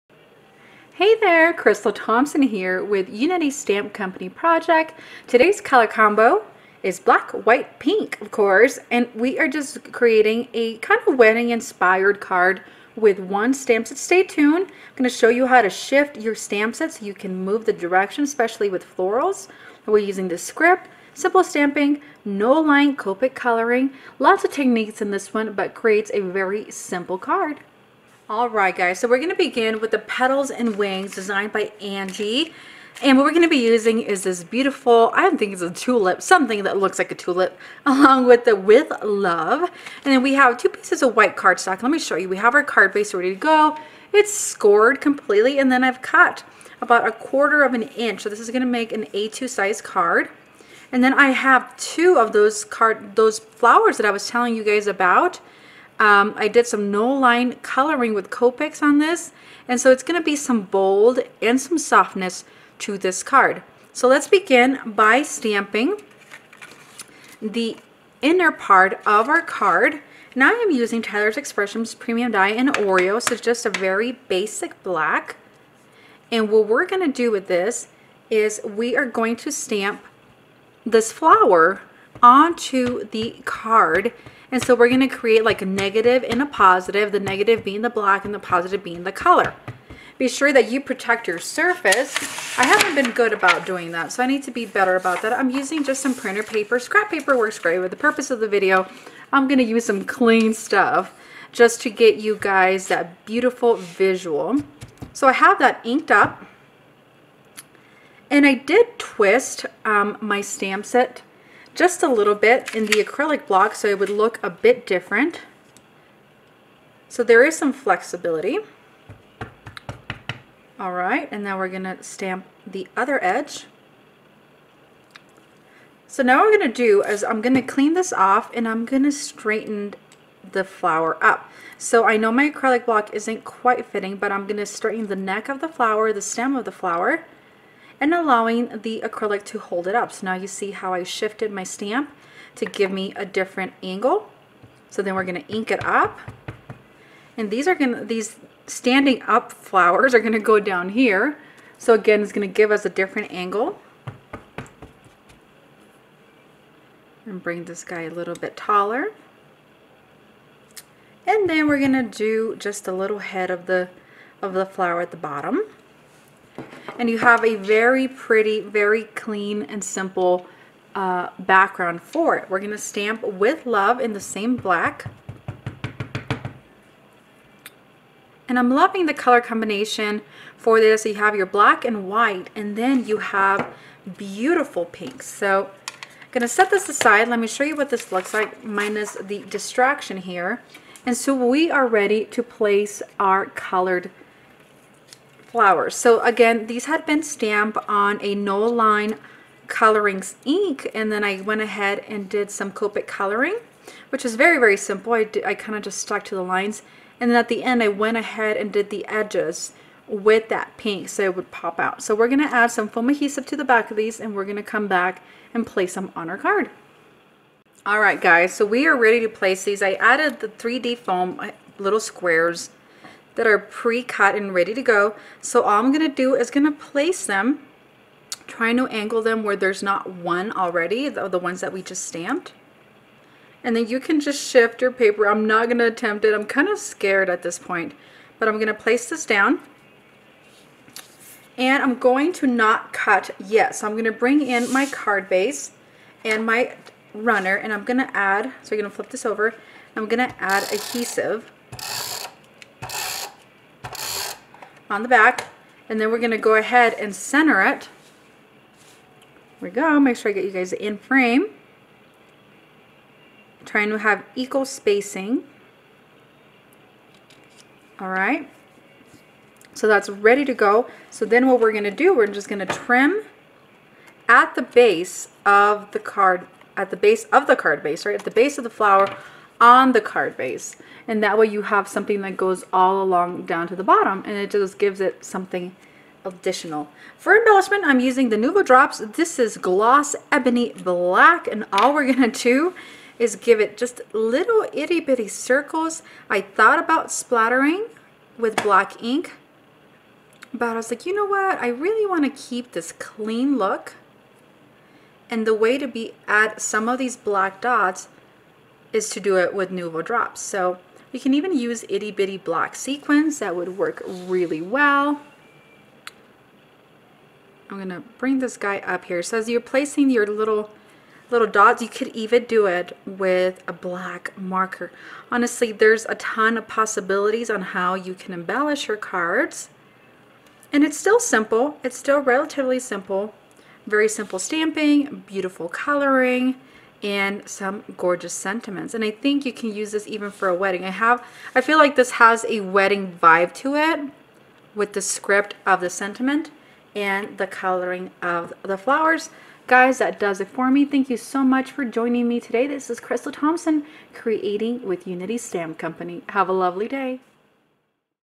Clean and Simple Pop Up Petals | One Stamp Set Card | How to Shift Stamps
Happy Tuesday! I have a fabulous color combination that is totally inspired by spring. We are using green, pink, black and white. I am creating this card with one stamp set from @unitystampco, shifting the stamp. See the full video tutorial on the YT channel.